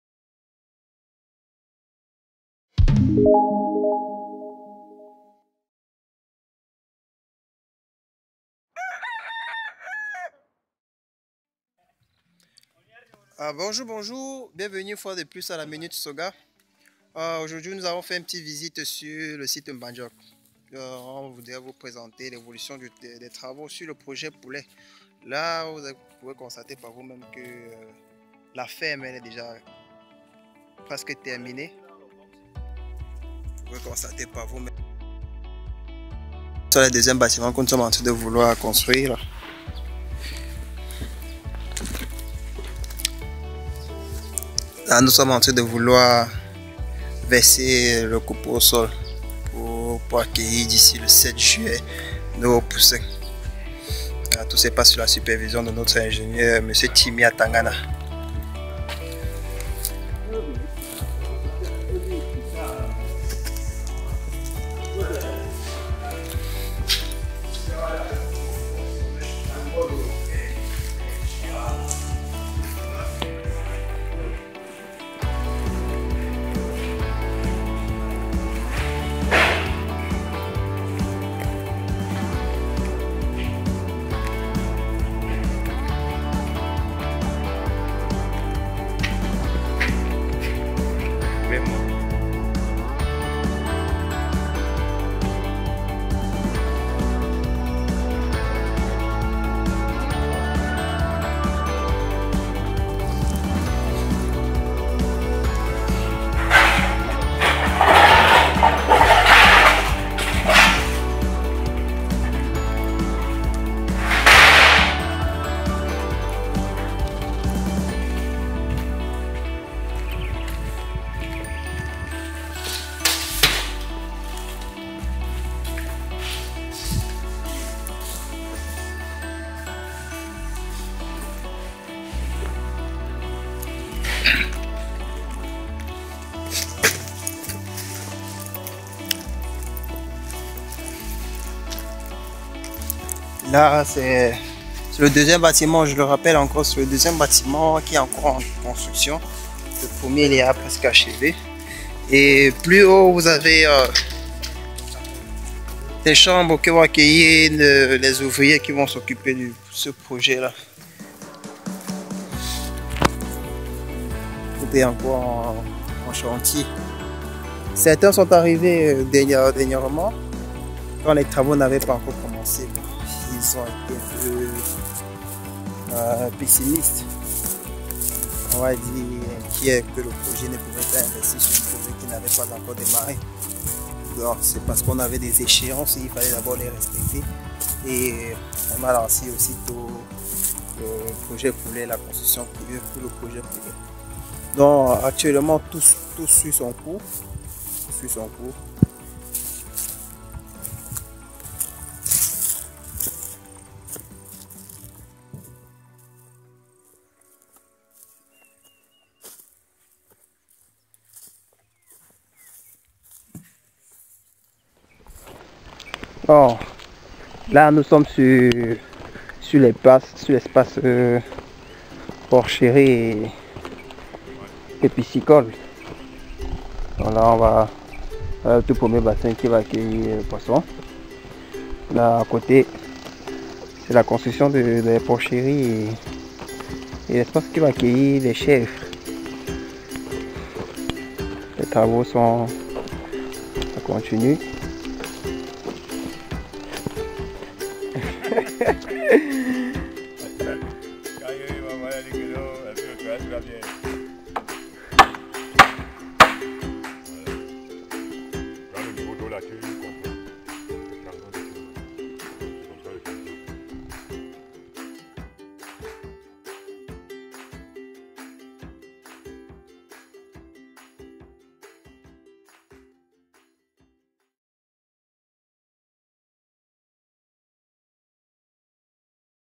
rapport projet poulet
évolution des travaux sur le projet poulet qui sera très bientôt lancé avec l'entrer des poussins en fin juillet 2020..